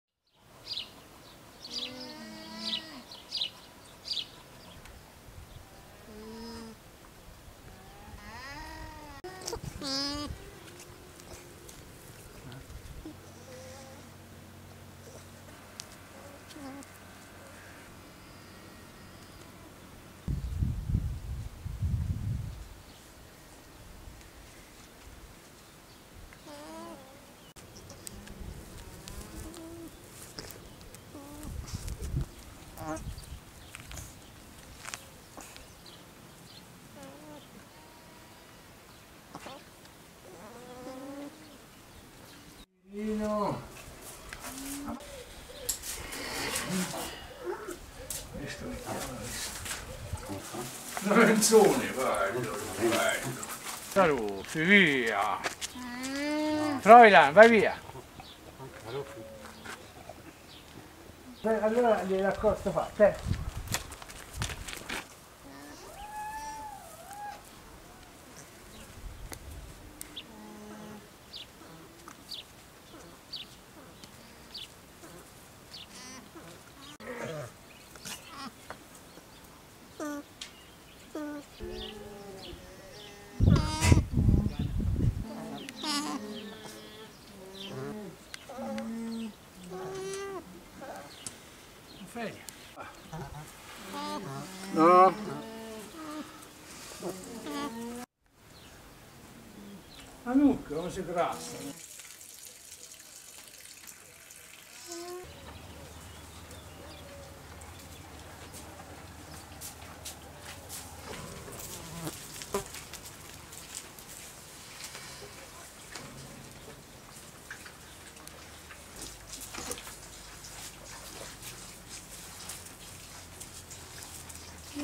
Alpaca Birth
This video is the moments after an Alpaca Birth. A newborn Alpaca gets an abrupt introduction to the world in the glare of the Tuscan sun. From a soggy start, he was already looking cute in his fluffy coat the next day.